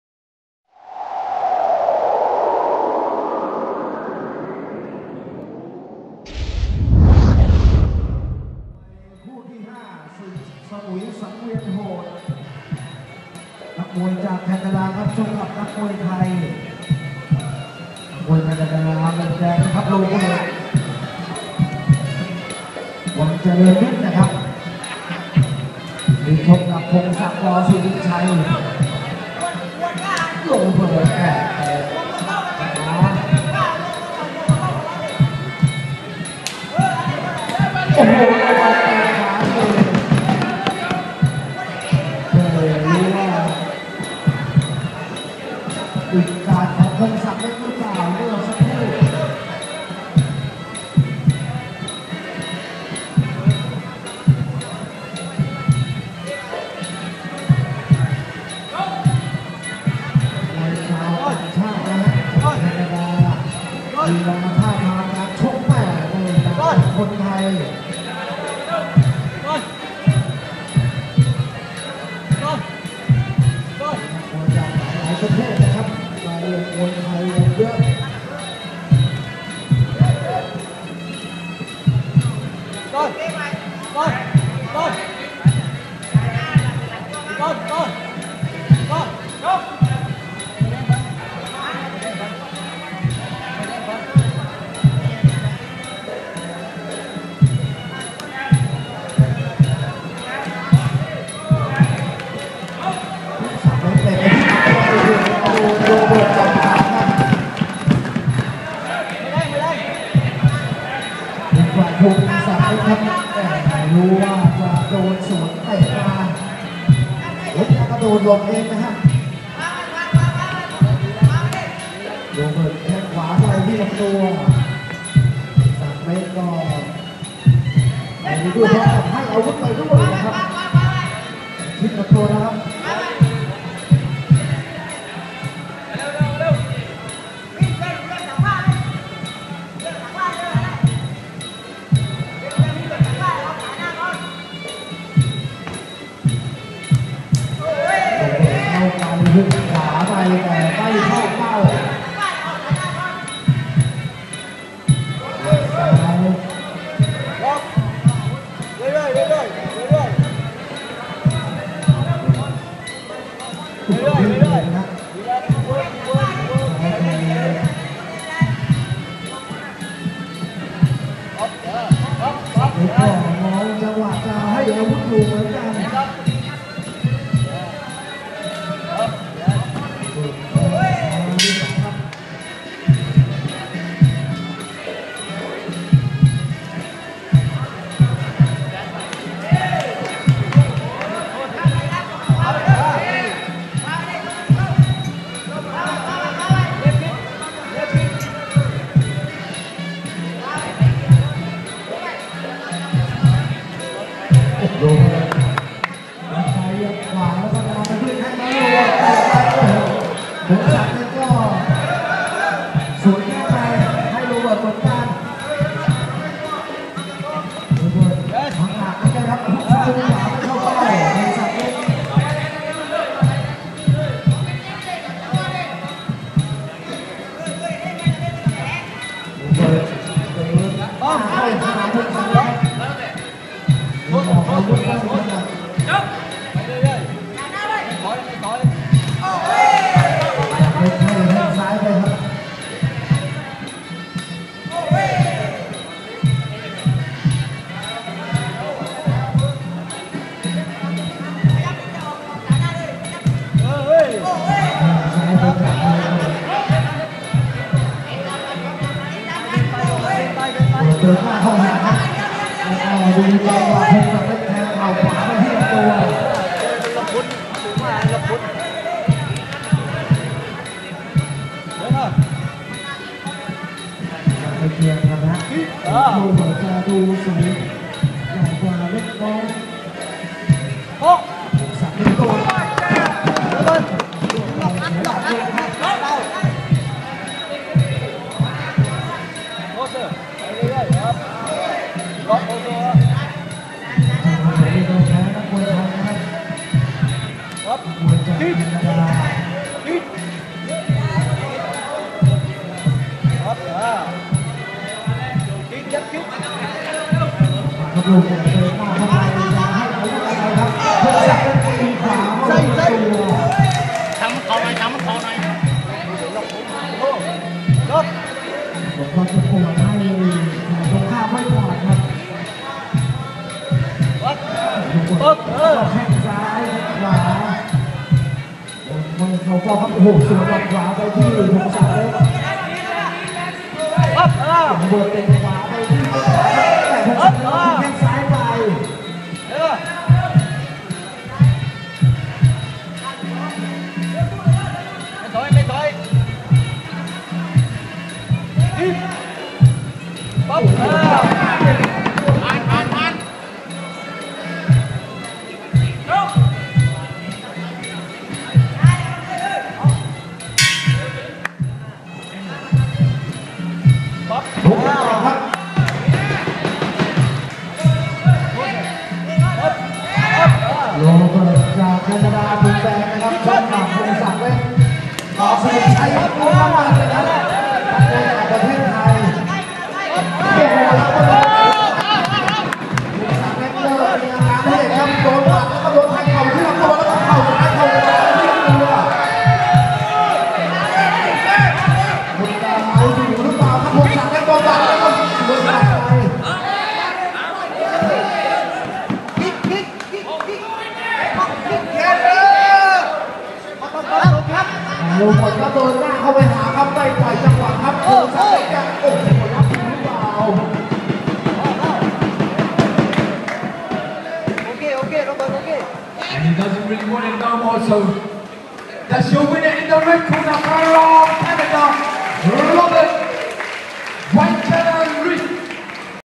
Robert Wan Chorenrit vs Pomsaklek Minatyuthin
Robert (red) vs Pomsaklek (blue) fight at Chaweng Stadium, Koh Samui, Thailand on May 10, 2013

Chorenrit Muay Thai Camp & Fitness Gym
214/9 M.1 Thongsala, Koh Phangan
Surat Thani, Thailand
84280